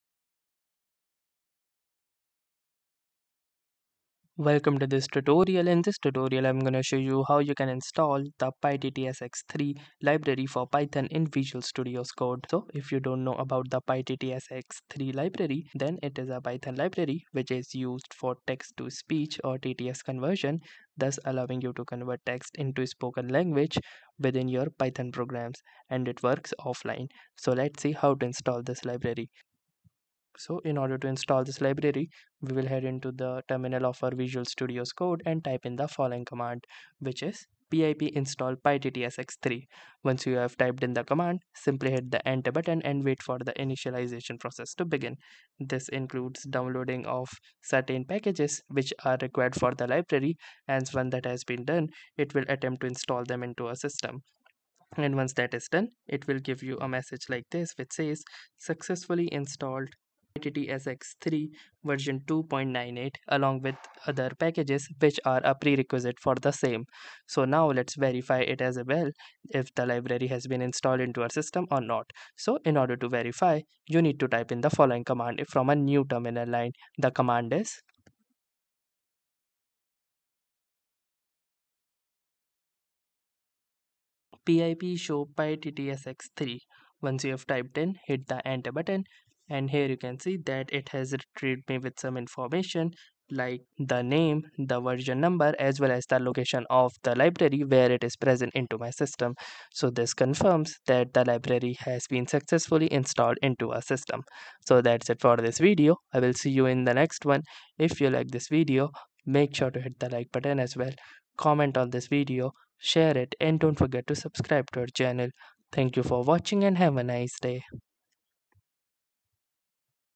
Install pyttsx3 Library in VS Code |Convert Text to Speech Easily for Python Projects | Quick Guide
Want to add text-to-speech (TTS) functionality to your Python projects? 🗣️ The pyttsx3 library allows you to synthesize speech using offline text-to-speech engines like Microsoft SAPI5, NSSpeechSynthesizer, and espeak. Whether you're building a voice assistant, audiobook generator, or accessibility tool, pyttsx3 makes it effortless!

🔹 Steps to Install & Configure pyttsx3 in VS Code:
1️⃣ Open VS Code & Navigate to Terminal
👉 Press Ctrl + ~ (Windows/Linux) or Cmd + ~ (Mac)
👉 Run the following command:
pip install pyttsx3

2️⃣ Verify Installation
👉 Check if pyttsx3 is installed successfully:
pip show pyttsx3

💡 What You’ll Learn in This Guide:
✔️ Install and set up pyttsx3 in VS Code
✔️ Convert text to speech using Python
✔️ Adjust voice settings for customization
✔️ Build voice-based applications
✔️ Use pyttsx3 for offline TTS processing